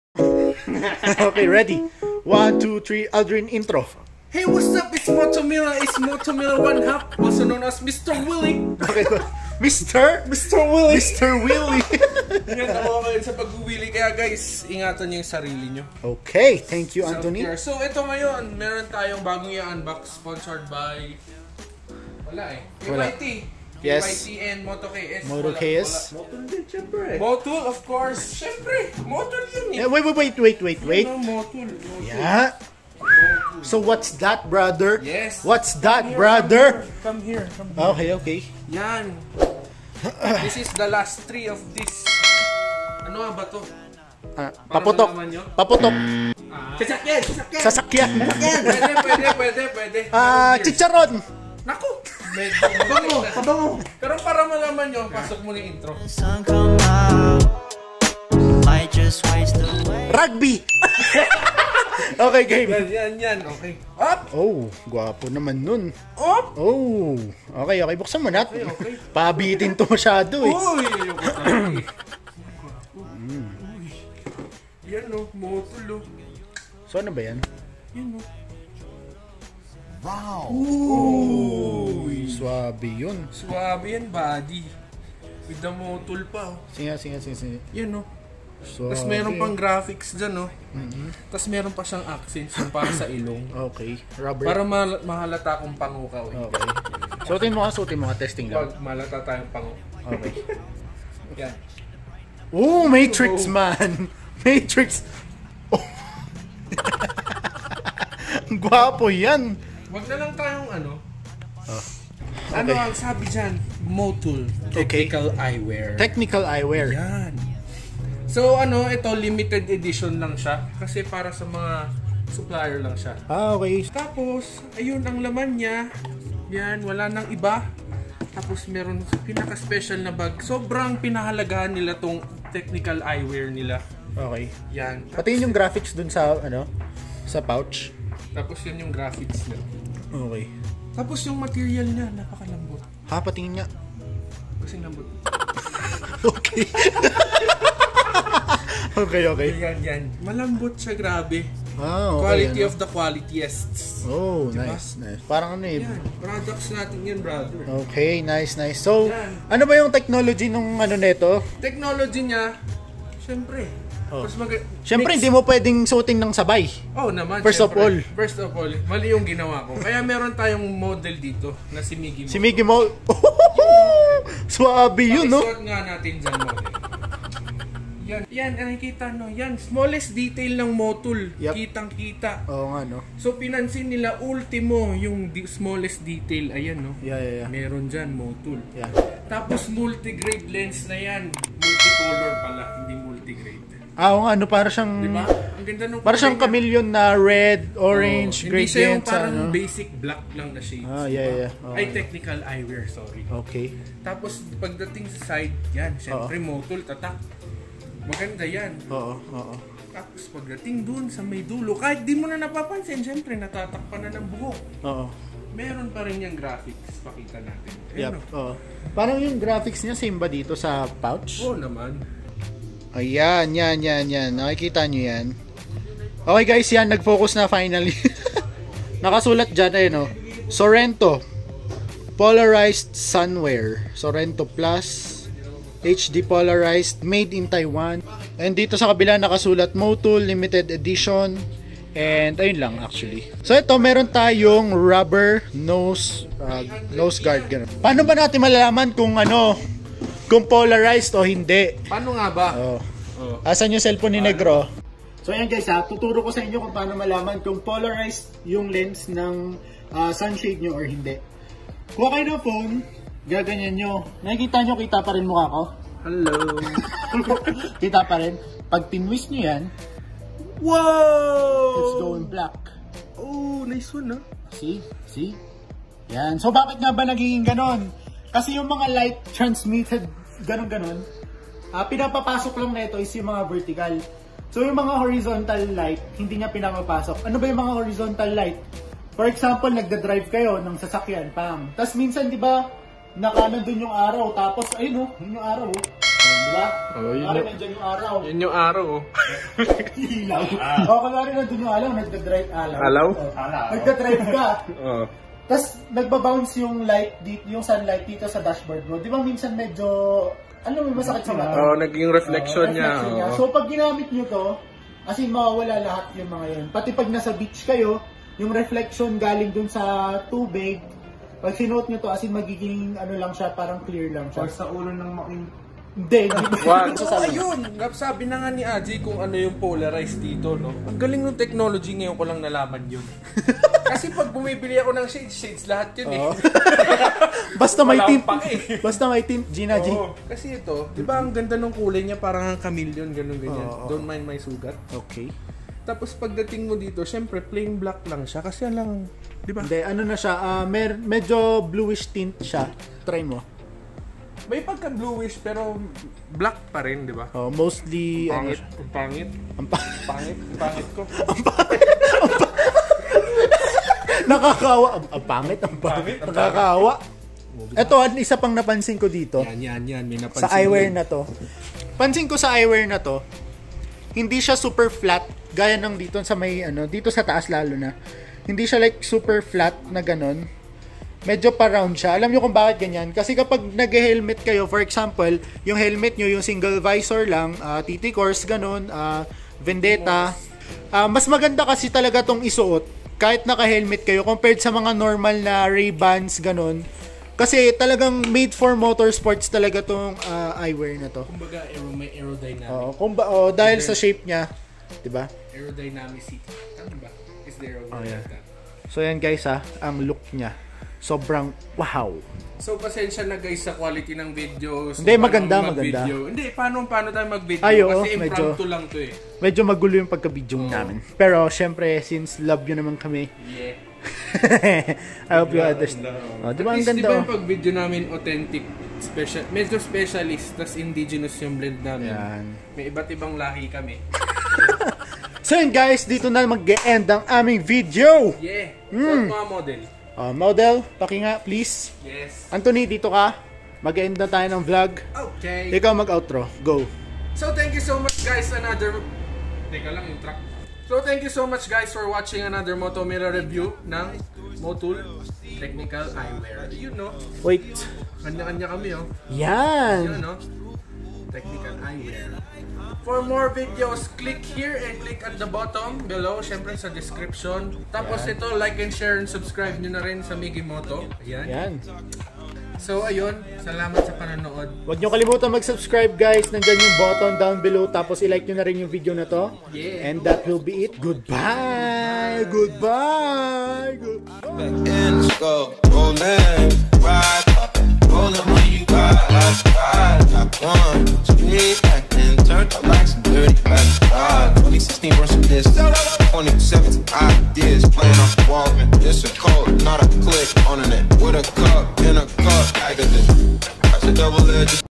MOTUL TECHNICAL EYEWEAR QUICK REVIEW: VLOG (tae content xD)
Welcome again to our channel!
Pagusapan natin ang MOTUL TECHINAL EYEWEAR / SUNGLASSES

This item is NOT FOR SALE. Marami ang nag tatanogn kung saan pwede mag avail ng Sunglasses na ito. This is exclusive to Motul dealers only.

We also distribute: YSS, KYT, Motul